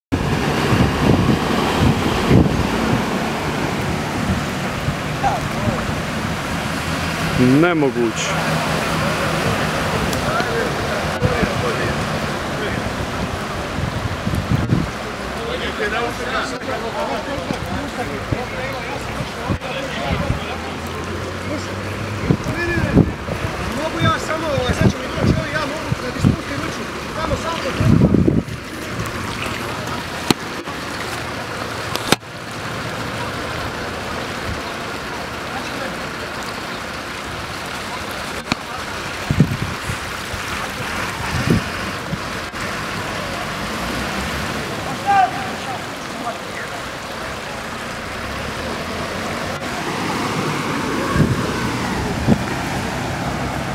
Nevrijeme - potopljen Split i Poljud 23.7.2011
U sat vremena u Solitu je palo 70 litara oborine po m/2 što je rekordna količina oborine u sat vremena. Poplavljen je velik dio Splita uključujući okolicu Poljuda.